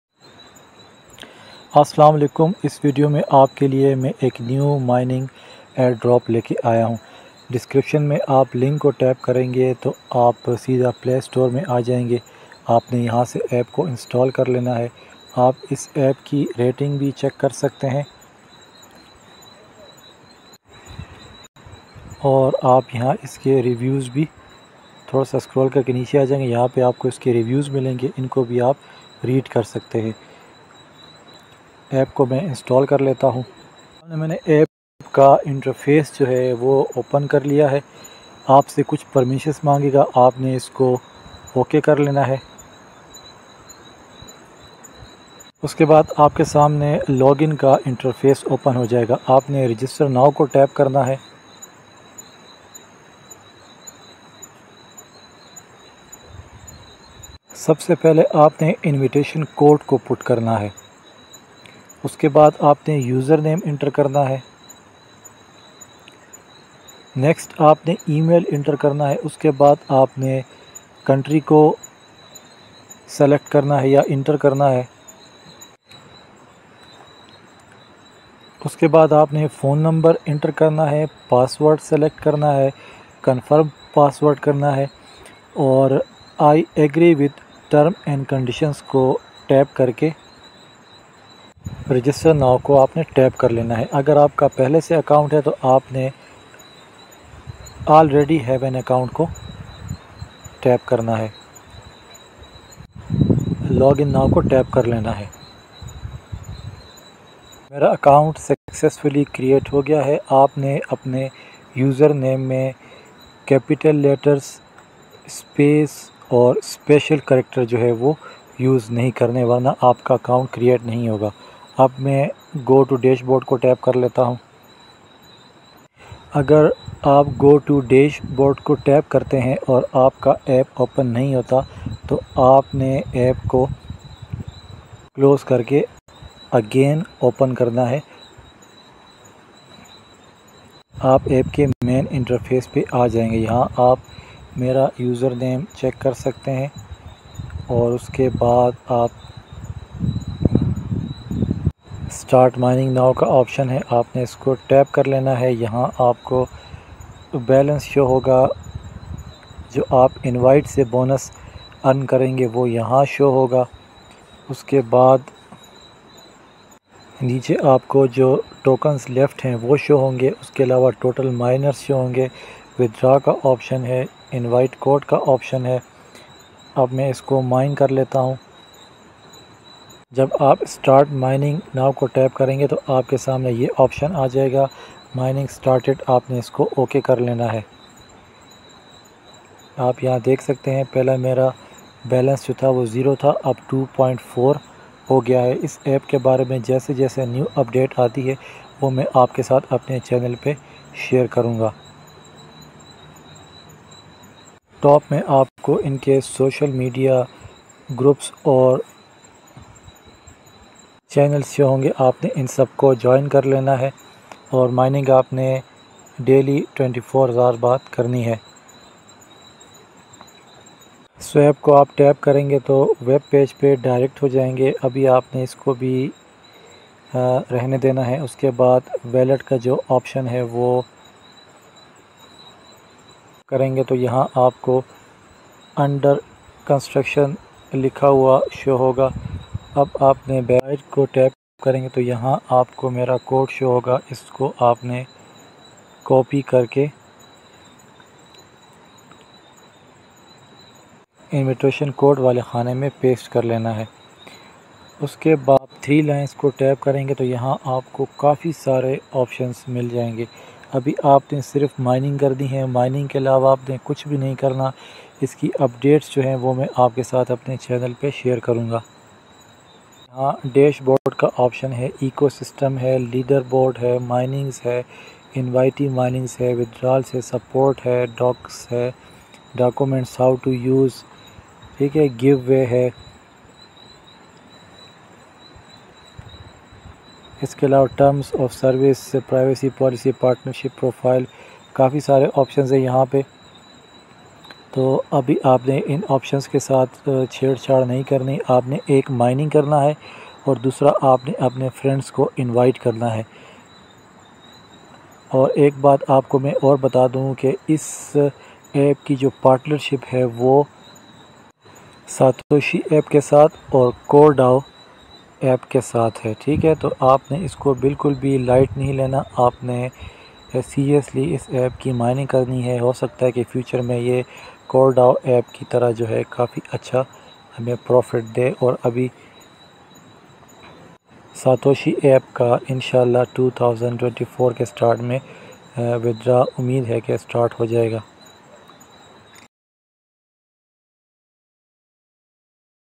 New Mining App Like Satoshi|Futuristic Airdrop|Technical Shahnavi
New mining app like Satoshi & CoreDao

Signup & start mining for free

Invite your friends and increase mining

support by Satoshi and CoreDao App

Invitation Code:
obaid131